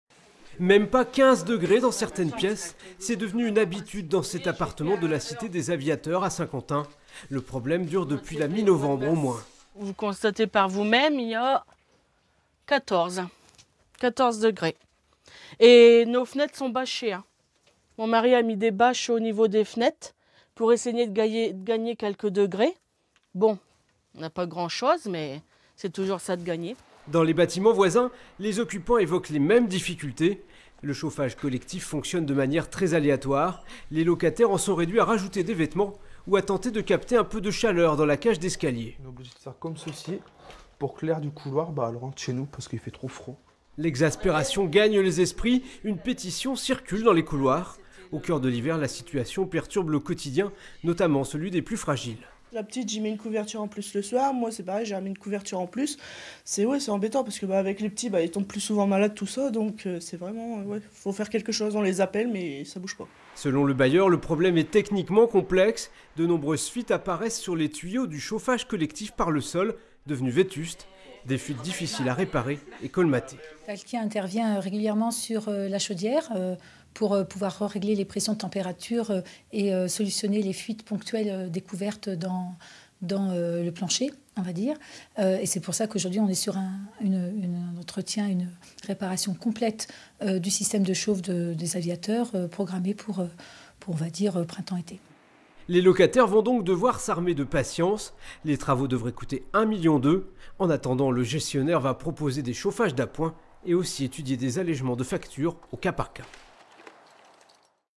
Des locataires privés de chauffage depuis novembre à Saint-Quentin
À Saint-Quentin, depuis plus de deux mois, de nombreux locataires de plusieurs immeubles de la "cité des aviateurs" sont confrontés à un chauffage défectueux. Résultat, il fait froid dans les appartements. Le bailleur reconnait les difficultés mais de gros travaux sont à prévoir et cela va prendre du temps.

Reportage de Rémi Vivenot, Eric Henry et Mathieu Krim
Intervenants : Delphine Delattre, Locataire ; Sophie Rasse, Locataire ; Mélanie Prévost, Directrice de la communication de "Clésence" - groupe Action logement